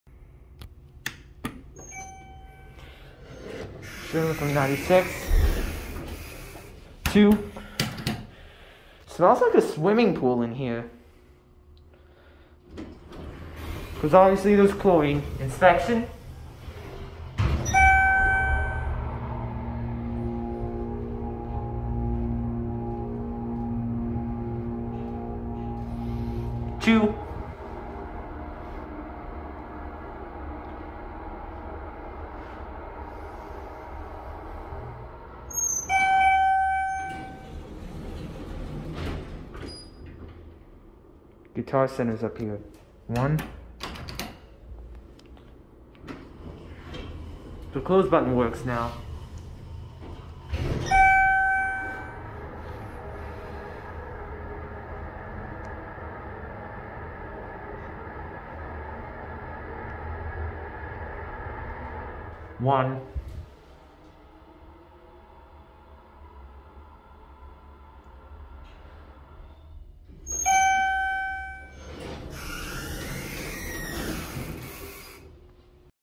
4K: 1996 Schindler 300A MT Hydraulic Service Elevator 8 | Redmond Town Center | Redmond WA
Located near Former Eddie Bauer/Guitar Center.

Taken 15 June 2021

Installed 1996